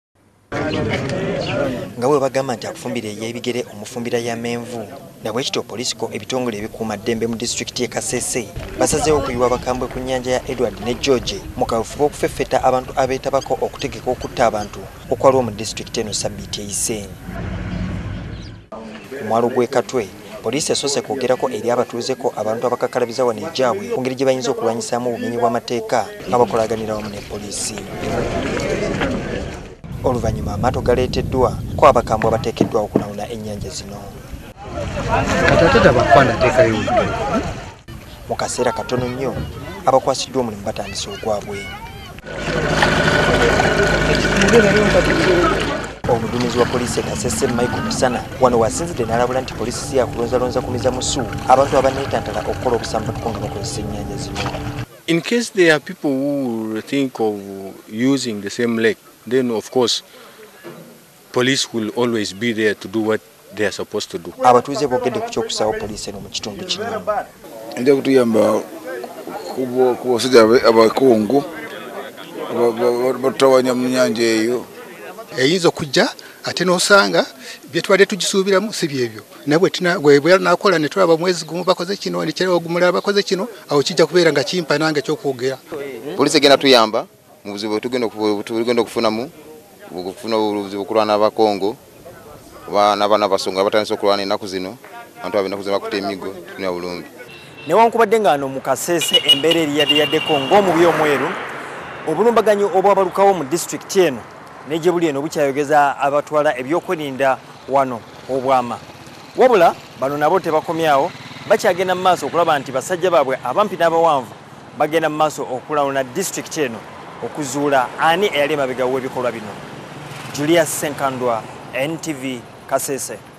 POLIISI ETUTTE ABASERIKALE KU BIZINGA
Abatwala eby'okwerinda e kasese,kati olutaalo lw'okufuuza abakola obulumbaganyi ku bantu abatali bamu,balwolekeza nnyanja eyitibwa Edward nga eno yeyawula Uganda ne Congo.

Poliisi ko UPDF,wetwogerera nga batandise okulawuna ennyanja eno eteberezebwa okuba nti oba olyawo,abajambula bakoze ebizinga byayo okutegeka ebikolwa byabwe